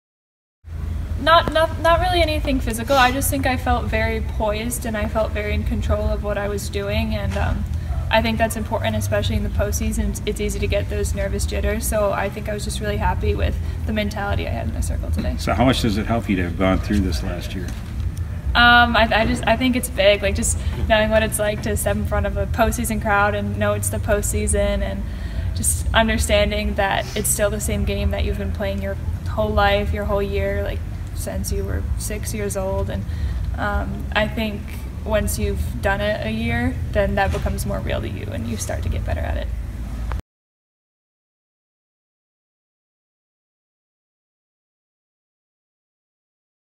NCAA regional: Michigan’s Meghan Beaubien throws 5 scoreless innings
Michigan sophomore pitcher Meghan Beaubien threw 5 scoreless innings in the team’s 8-0 win over St. Francis in NCAA regional opener.